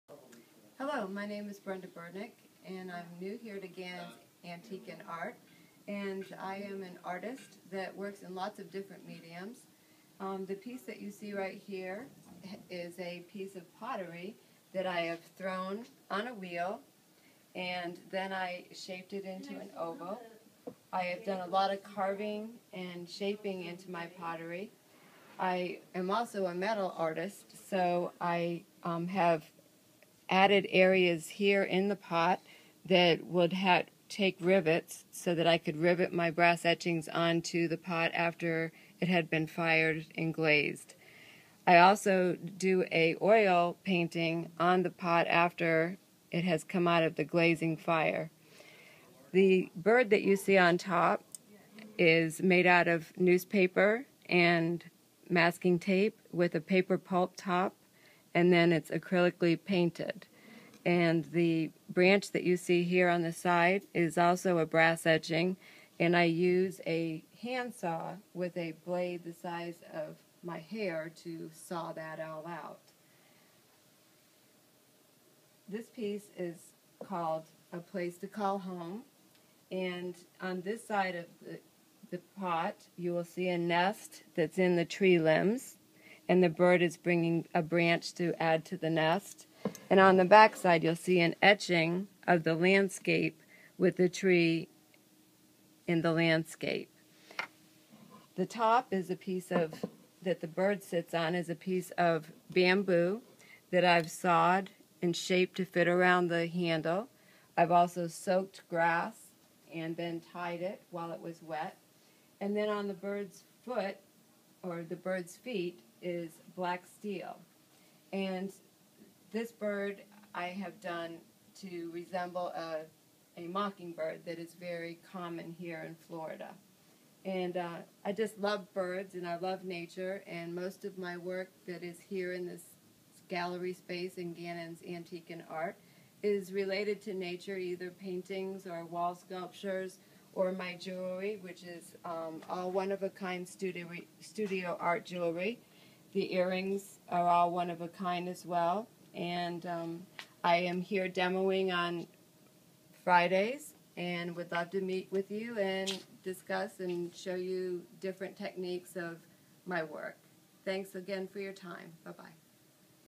ART GALLERY, Studio Art Pottery Sculpture Mixed Medium
Discover one of a kind Art pottery, mixed medium sculptures in the art gallery in our antiques Mall. Unique treasures including miniatures, copper etching, paintings, one-of-a-kind jewelry and so much more! This art gallery has very unique takes on traditional media. It is not your ordinary art!